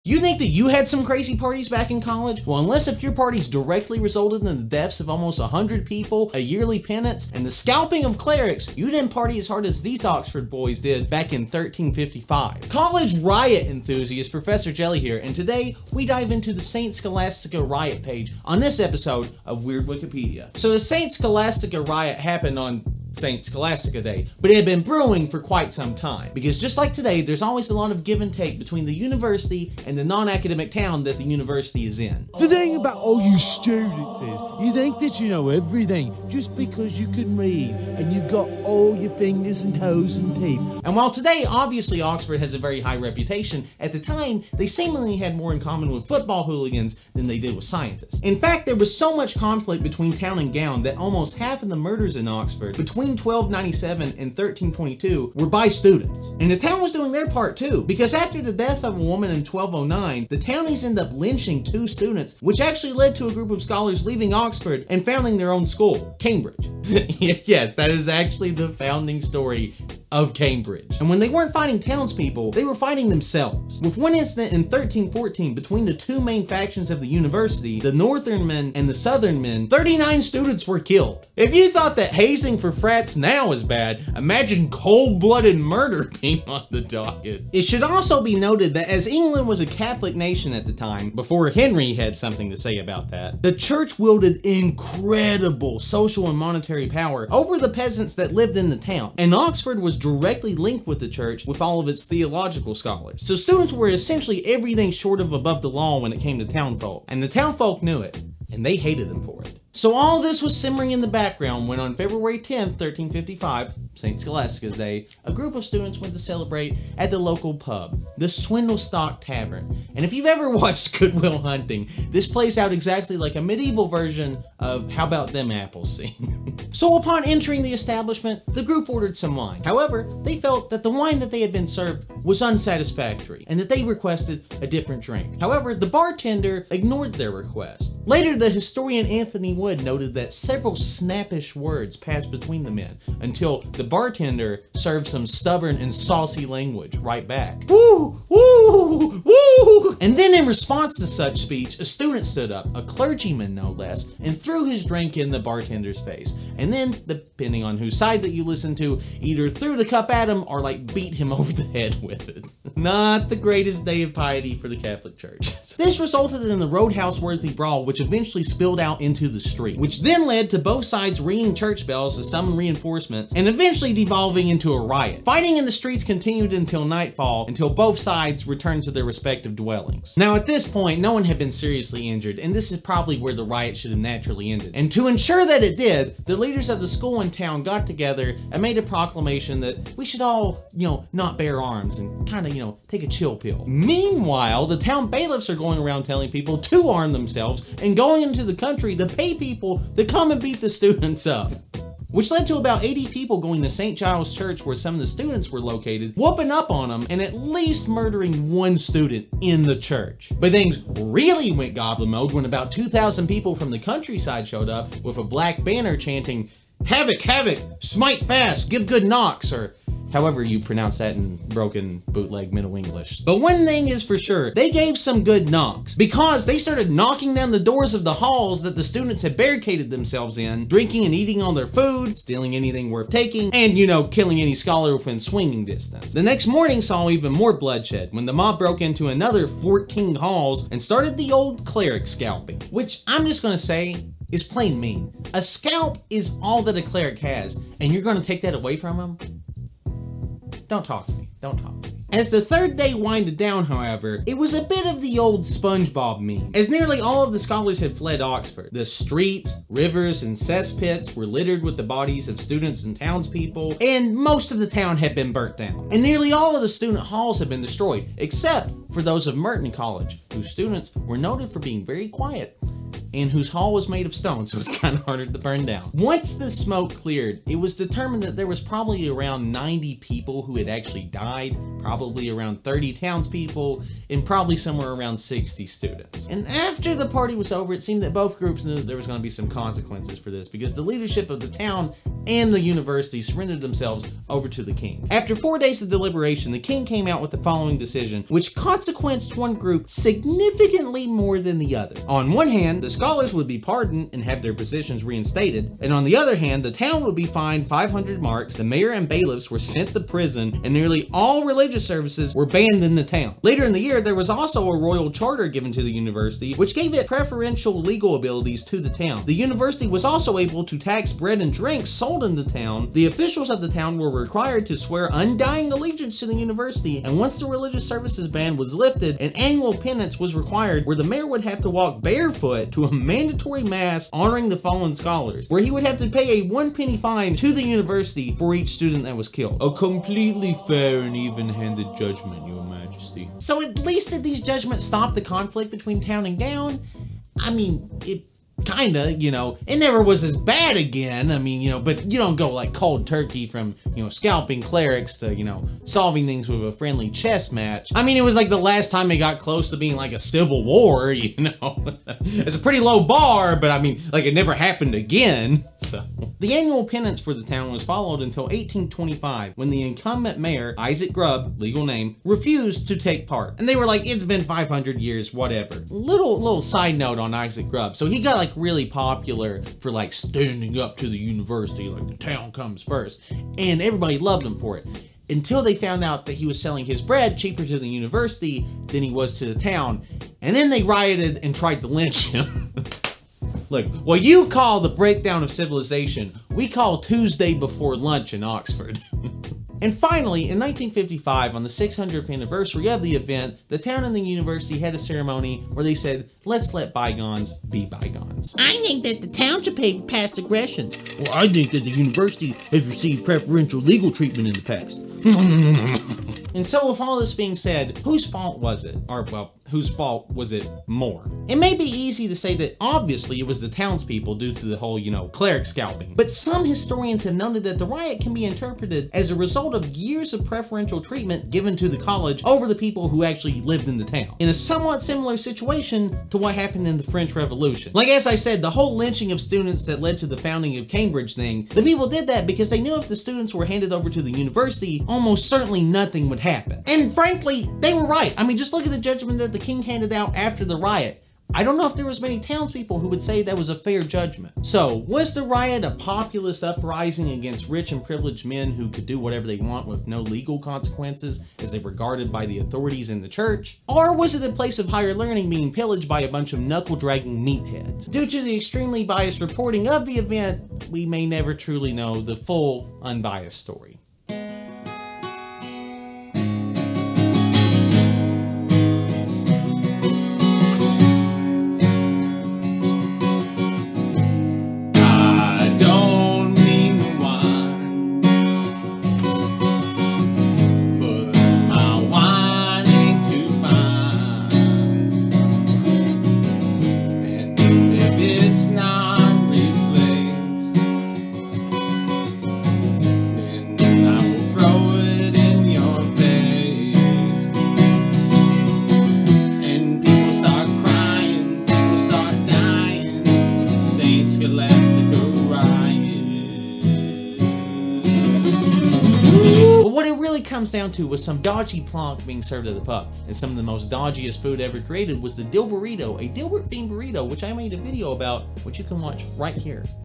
What Was The St Scholastica Riot EXPLAINED
In one of the worse cases of bad wine, a glass of dodgy wine indirectly resulted in a college riot in Medieval England. In this episode of Weird Wikipedia, Professor Jelly explains the St. Scholastica Riot page on Wikipedia.